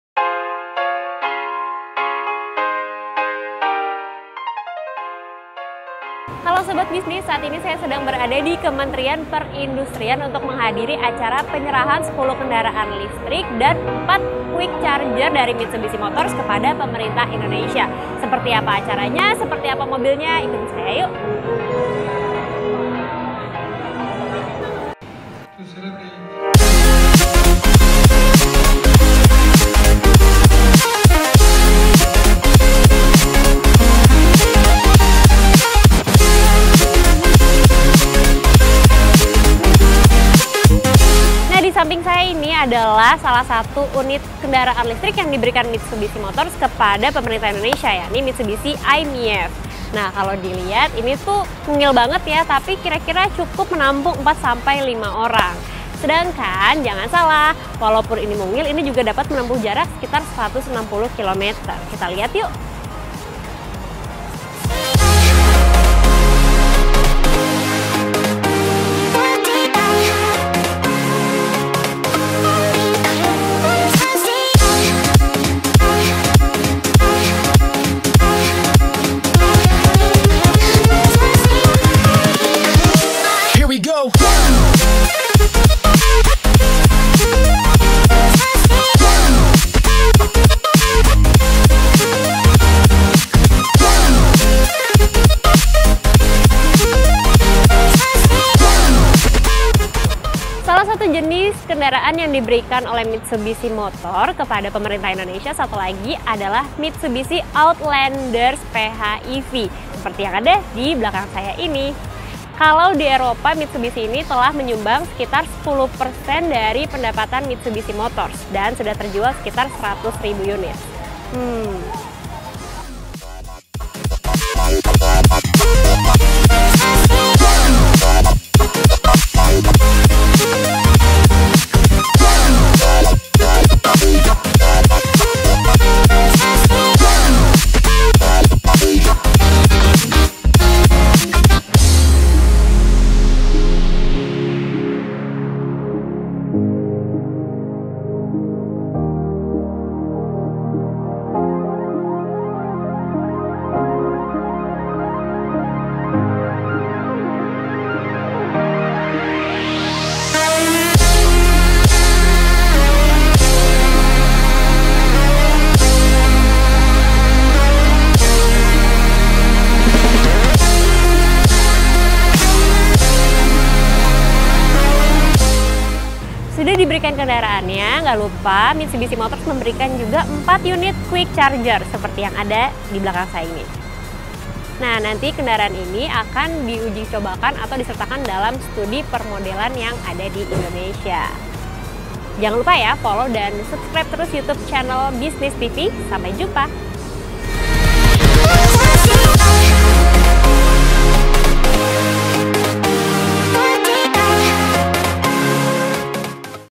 Mengintip Mobil Listrik Mitsubishi
Sebanyak 8 unit Outlander Plug-In Hybrid Electric Vehicle (PHEV) dan 2 unit i-MiEV telah diserahkan oleh CEO Mitsubishi Motors Corporation Osamu Masuko kepada Menteri Perindustrian Airlangga Hartarto. Seperti apa tampilan mobil listrik Mitsubishi?? Yuks kita intip ulasannya pada video berikut ini.